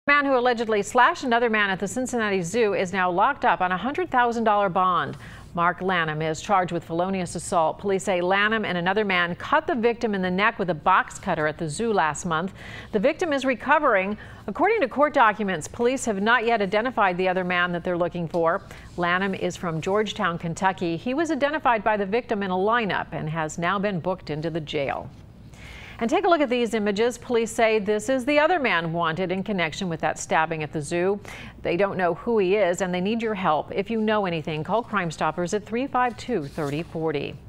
Police arrest suspect in zoo slashing
CINCINNATI (WKRC) -  Police have arrested a man suspected of slashing another man at the Cincinnati Zoo. Mark Lanham is charged with felonious assault.

Police say that Lanham and the victim got into an argument at the zoo on July 29. Police say Lanham and another man attacked the victim, stabbing him across the neck. Police have not identified the second suspect.

Assistant prosecutor David Wood showed the judge a picture of the victim's injuries. In addition to being sliced across the neck, Wood said he was beaten in the face.

Lanham is from Georgetown, Kentucky. He was booked into the jail Tuesday after the victim identified him in a lineup.

Lanham's attorney says he turned himself in. The attorney called the case sensational and said that Lanham did not have the knife. He said Lanham only got involved to try to stop the incident.

A judge ordered Lanham held on $100,000 bond.